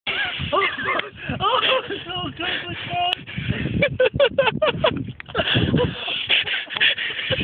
Fit of laughter!!
A laughing fit On top of the ridge of Crib Goch!  Ouch!!!